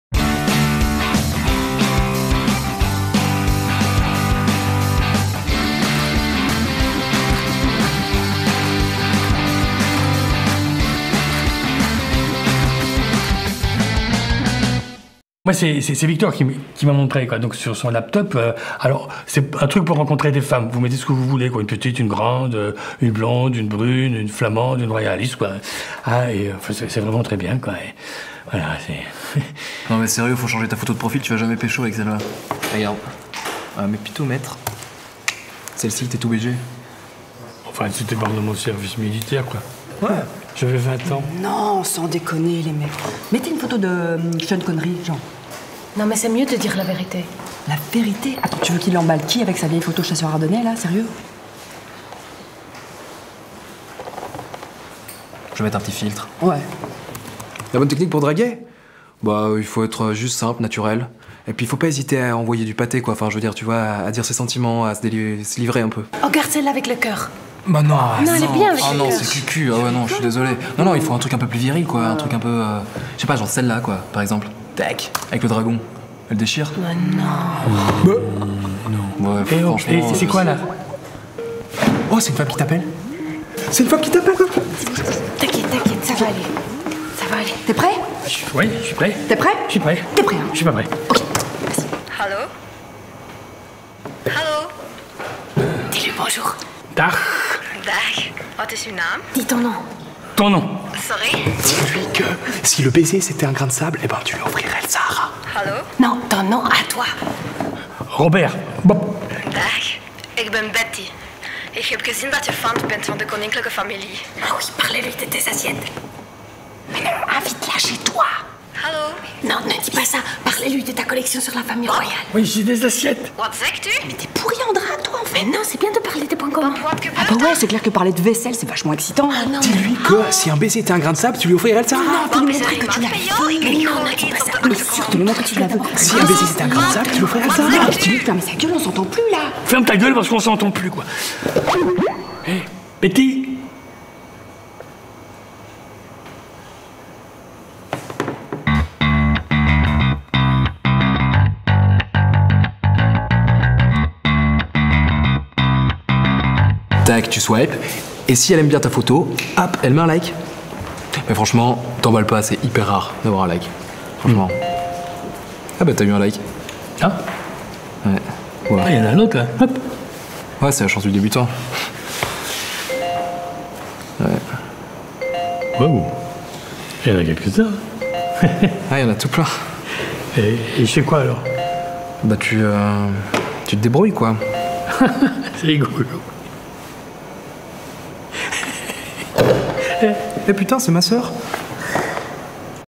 Promo2017 SAISON 1 - E02 : Le site de rencontre
EPFC, Enseignement de Promotion et de Formation Continue de l'ULB et de la CCIB.

avec  Alexia Depicker, Amel Felloussia,  Emilien Vekemans et Jean-Louis Sbille